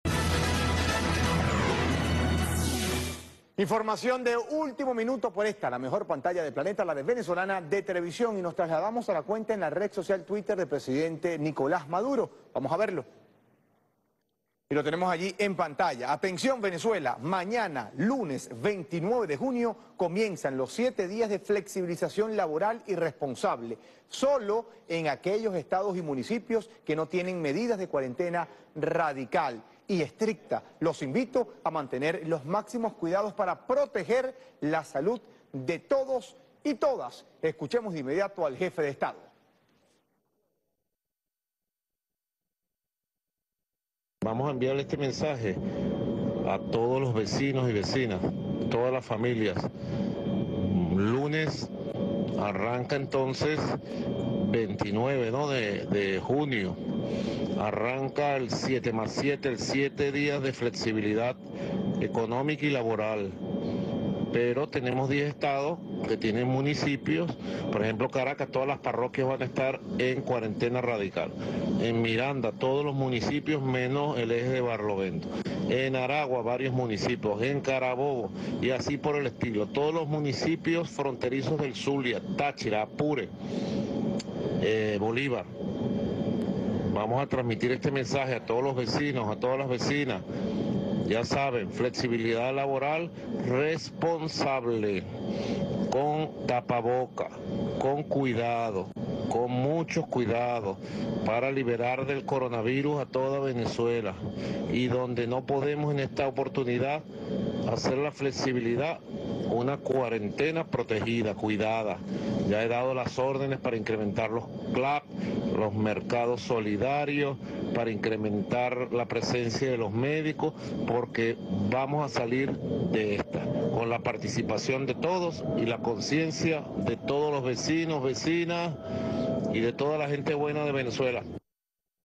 Maduro llama a acatar flexibilización y cuarentena por Covid-19 desde este lunes 29
El Presidente de Venezuela, Nicolás Maduro, hace un llamado a acatar la cuarentena radical el 12 estados del país, y la flexibilización en los demás estados, que inicia este 29 de junio de 2020